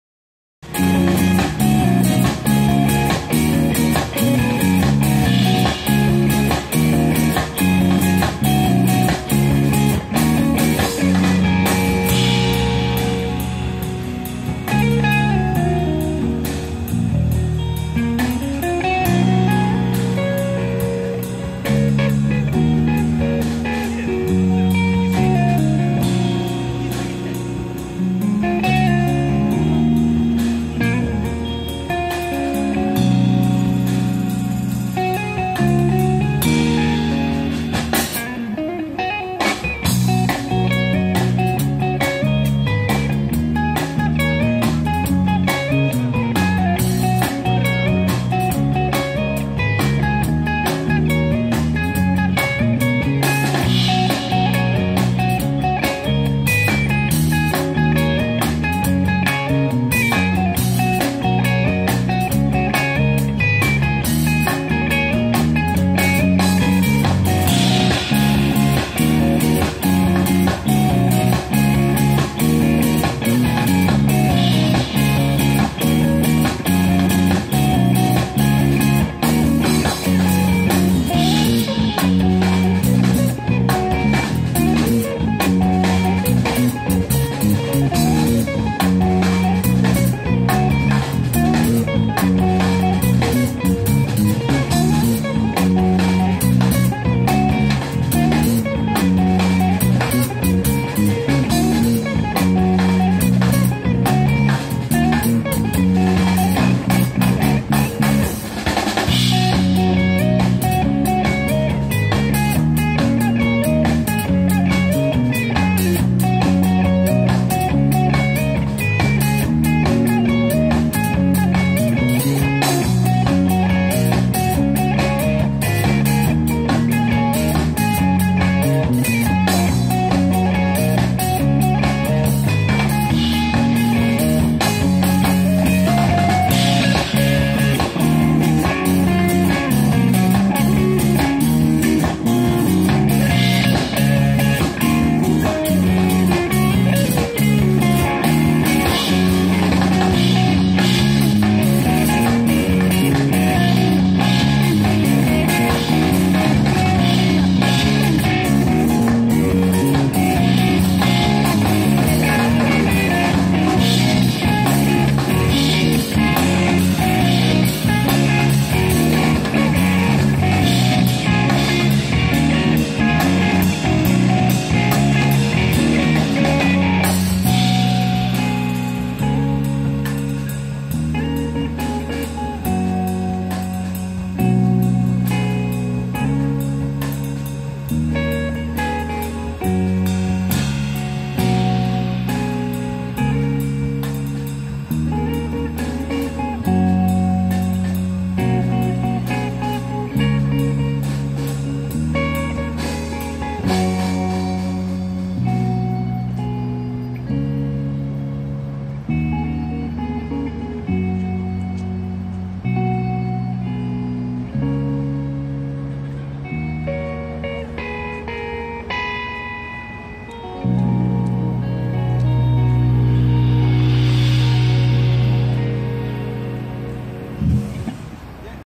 Street Performers in Namba Station
Band lokal menggelar pertunjukan jalanan di sekitar Stasiun Namba Osaka dalam rangka promosi mini album